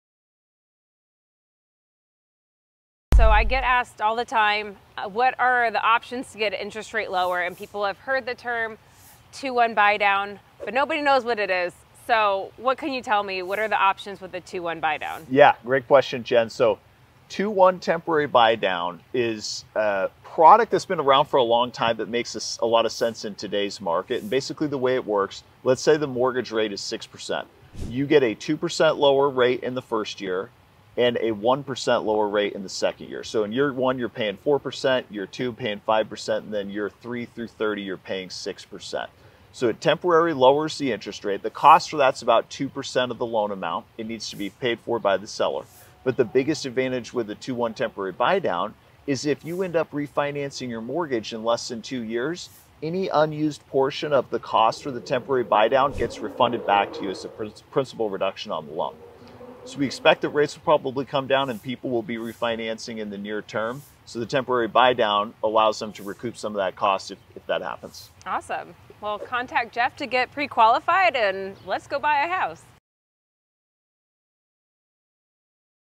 💰 FINANCE FRIDAY 💰 Money Saving Mortgage Hack: 2-1 Buy Down
💰 FINANCE FRIDAY 💰

With our preferred mortgage broker, Jeff Smith, CEO of Tiger Home Loans & Jennifer Marsh, team lead for The Marsh Team

🤔 Ever wondered how to snag a lower interest rate on your mortgage? Well, here's the scoop! The 2-1 buy down lets you enjoy reduced rates for the first two years. In year one, you pay 4%, then 5% in year two, and it goes back to the standard 6% after that.

The best part? The seller foots the bill! Yep, they cover the cost of lowering your interest rate, which is around 2% of the loan amount. 👀

🍒 And here's the cherry on top: if you refinance within two years, any unused portion of the cost is refunded to you as a principal reduction lump sum. So, not only do you save money upfront, but you might get some cash back too!

So, next time you hear about the 2-1 temporary buy down, remember it's a savvy way to score a lower interest rate. Don't miss out on this mortgage magic! 🏠

👩🏻‍💻 Jennifer Marsh - The Marsh Team

Jeff Smith - Tiger Home Loans